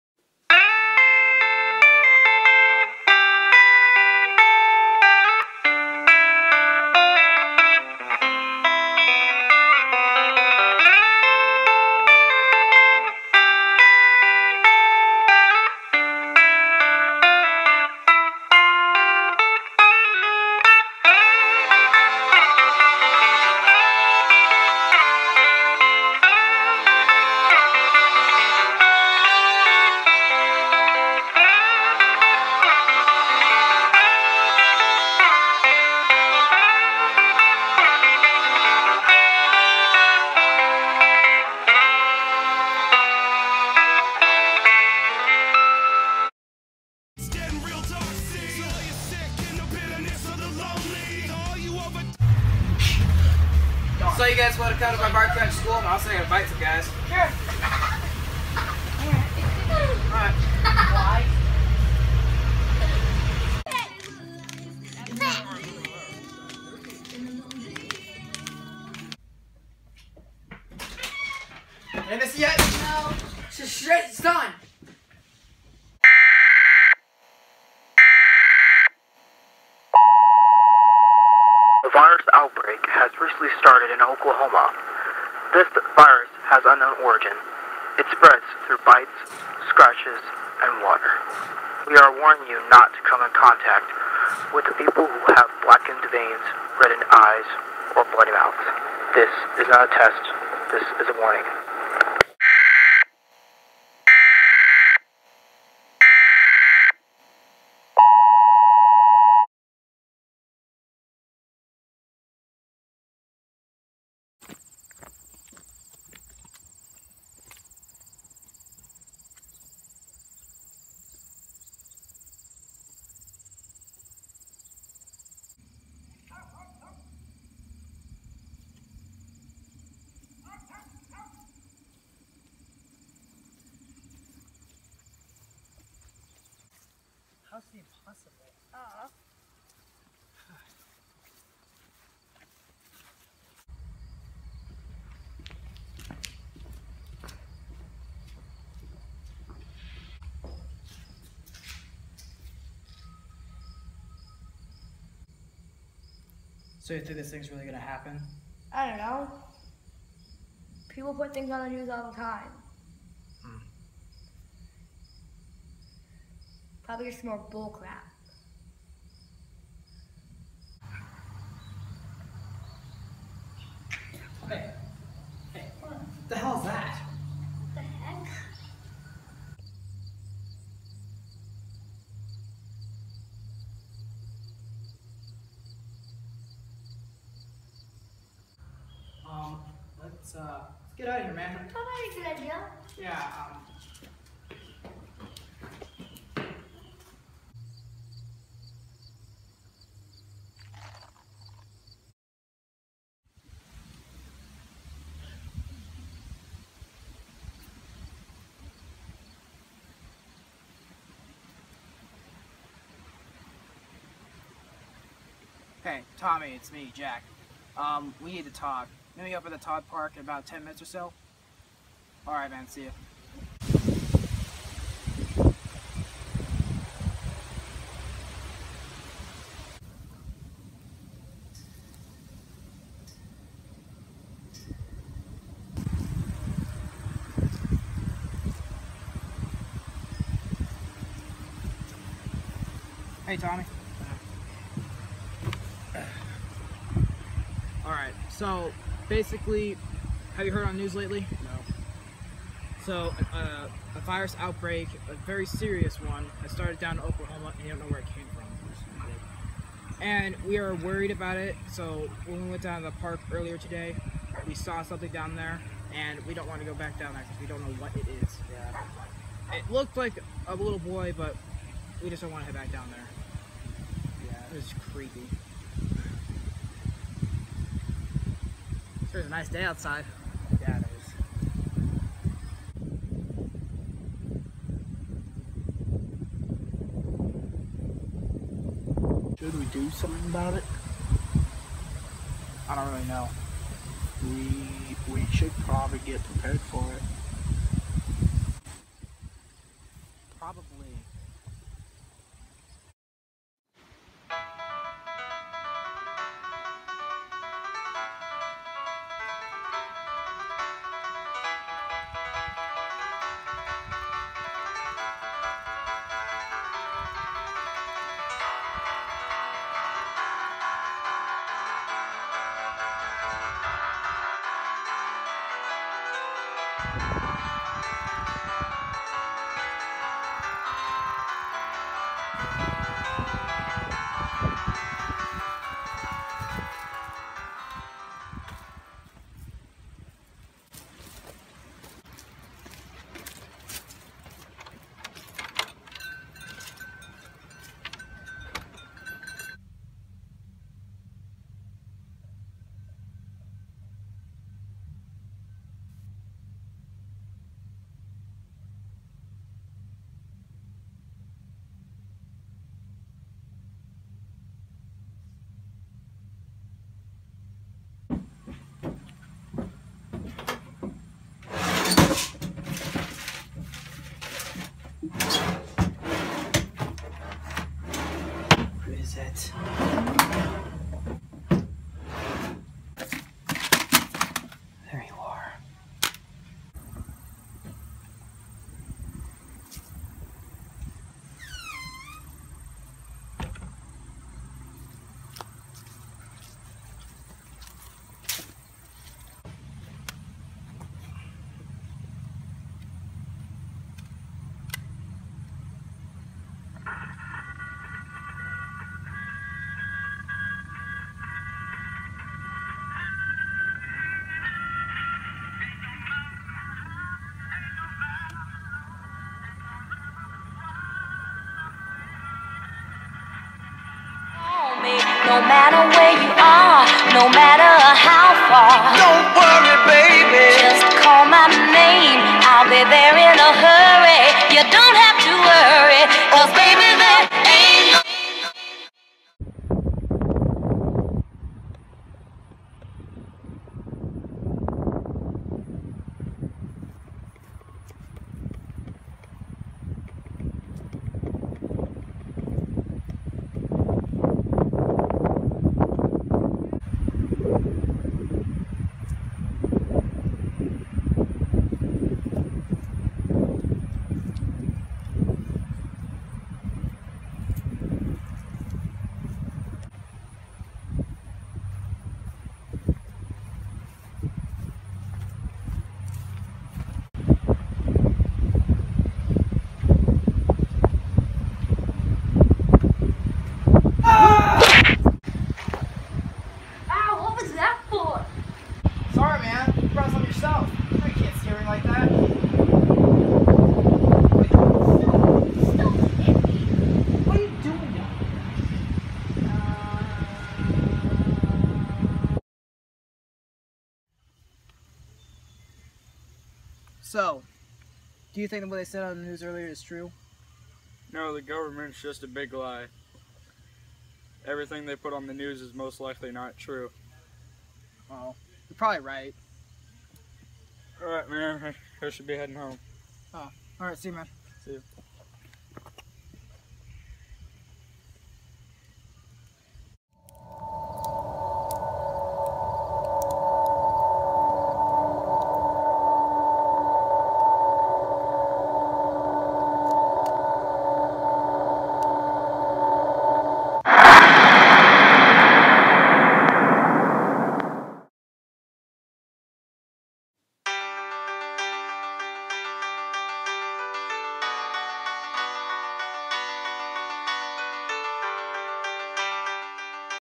Amongst the Dead S1: E1 TEST
This was meant to be more serious. The basic story of this episode is that Jack and his friends know of the virus and he doesn't know what to do about it. Comedy will be left out after this. Enjoy.

I DO NOT OWN, any of the music used in this video. Please refrain from thinking it is mine.